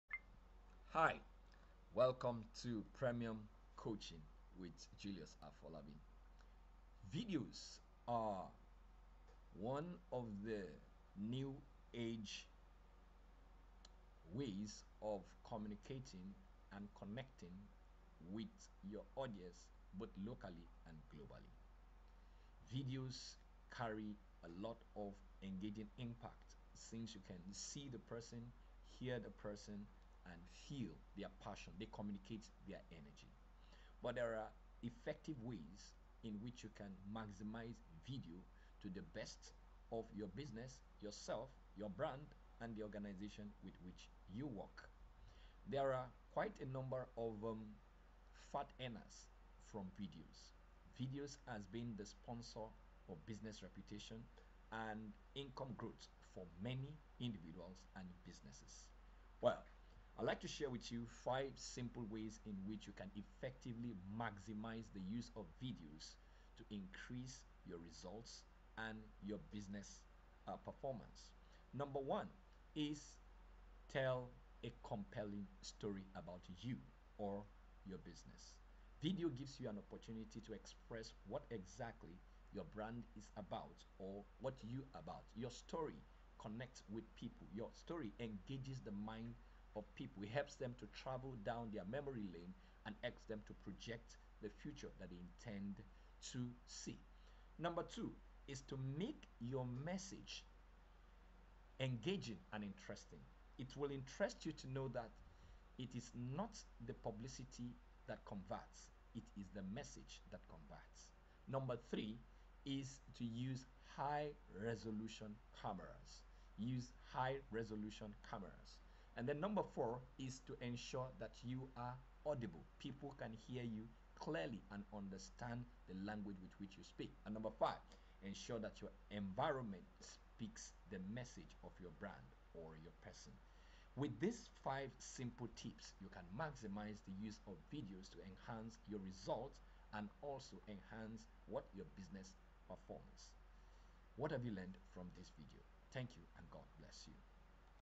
Five (5) Simple Ways to Effectively Use Videos to influence Your Business Results
As Attention Span dwindles, Videos have become the NEW GOLD in securing the attention that creates conversations that influence The Realization of Our personal and Business Goals.